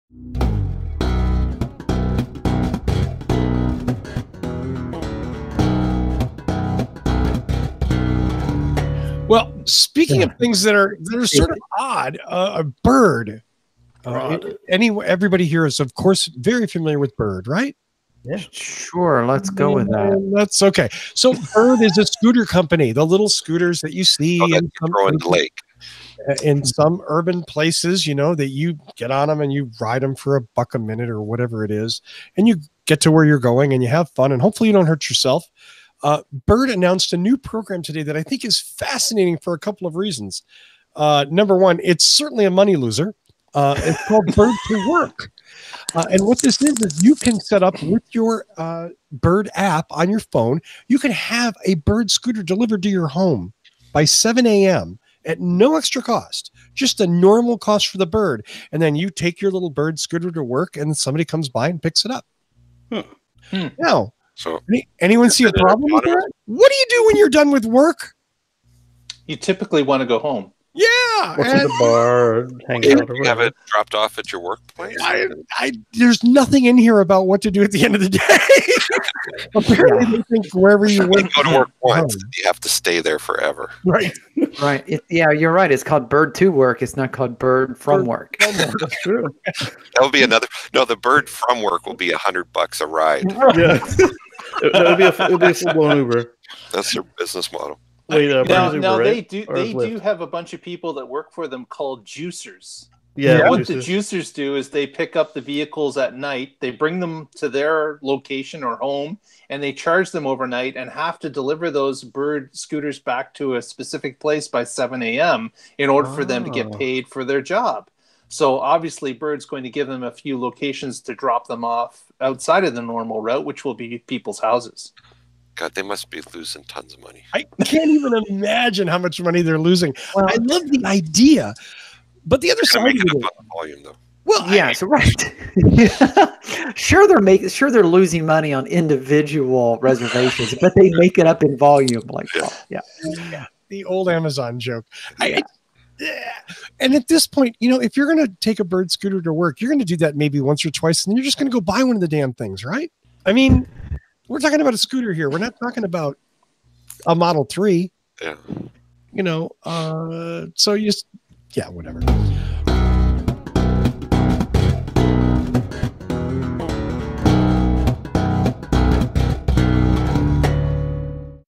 WDU Short - 314.4 - Bird to Work is as Stupid as it Sounds
Bird Scooters wants to rent you a ride to work.  There's oddly no mention of how you're going to get home.  And just the beginning of how bad an idea this is.

Youtube.com/WhatDrivesUs